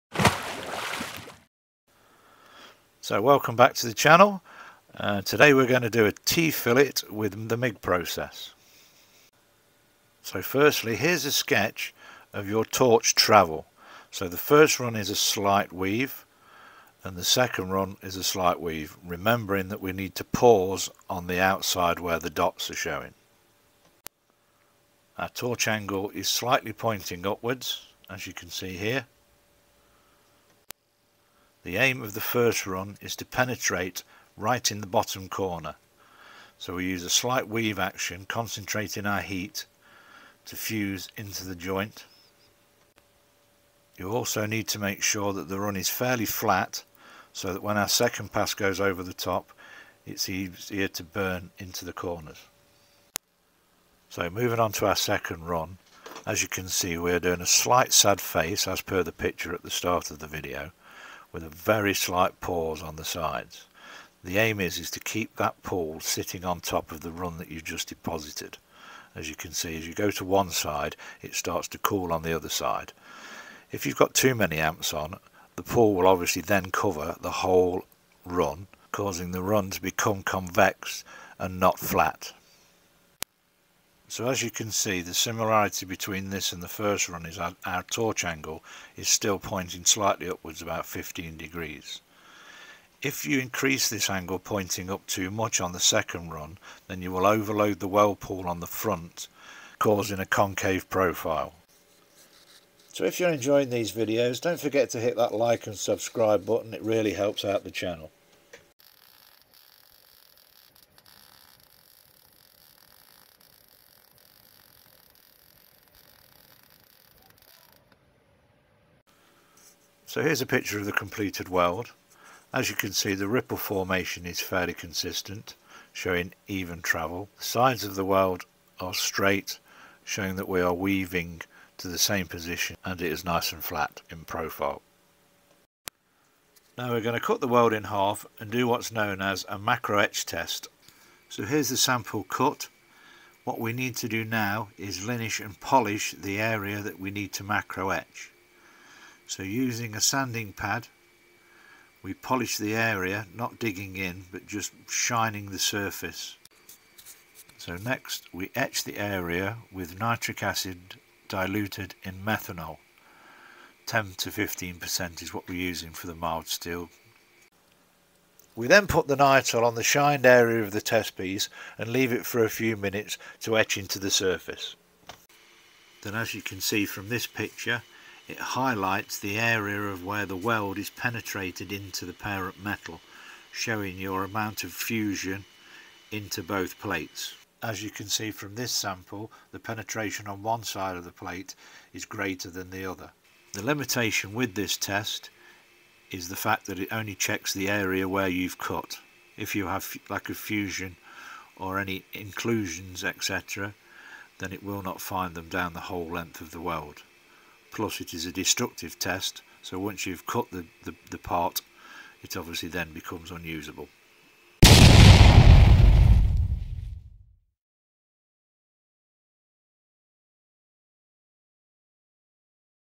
MIG Welding Tips & Tricks: How to Achieve Perfect Welds
MIG (Metal Inert Gas) welding vertical is a welding process used to join two pieces of metal along a vertical joint. It is commonly used in industries such as construction, shipbuilding, and metal fabrication.

During MIG welding vertical, a consumable wire electrode is fed through a welding gun, and an electric arc is created between the electrode and the workpiece. The heat generated by the arc melts the metal at the joint, and the molten metal solidifies to form a weld bead.

To perform MIG welding vertical, the welder must maintain proper torch angle, travel speed, and distance from the workpiece. The electrode should be pointed slightly upwards, and the welder should move the gun in a weaving motion to ensure proper fusion of the metal. The welder must also ensure that the weld pool remains stable and does not sag or drip down the joint.

MIG welding vertical can be used to join a variety of materials, including steel, stainless steel, and aluminium. It is a relatively fast and efficient welding process that can produce high-quality welds with minimal distortion. However, it requires skill and experience to produce high-quality welds, and proper safety precautions should always be taken when welding.

Vertical welding joints are commonly encountered in construction and shipbuilding, where upright welding is necessary. With proper training and experience, high-quality welds can be produced using MIG welding vertical.

Mig welding in the vertical PF position using 18 volts and 4mpm wire speed.